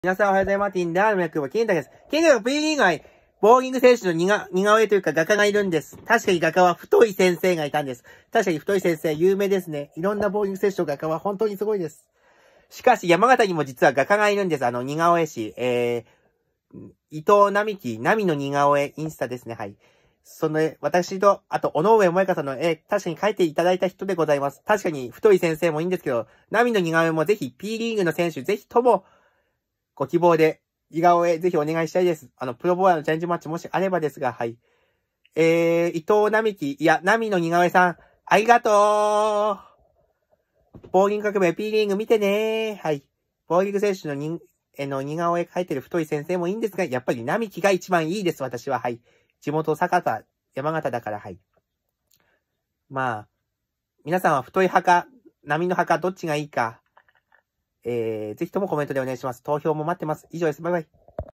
キングたくのYJ#21614
キングたくのpリーグ愛#1157。ボウリング選手画家の話。